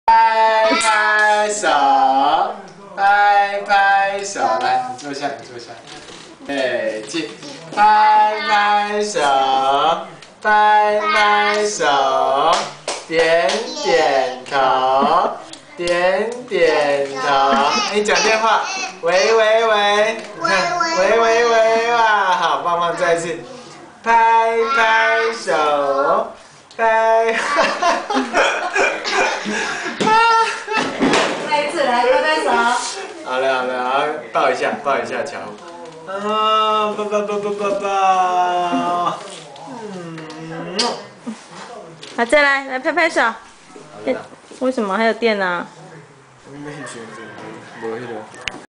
辰與巧虎
這次辰辰到上海,我發覺他對巧虎也很有興趣,今天晚上趁著飯後,替她拍了段跟巧虎互動的影像.
請大家慢慢欣賞喔!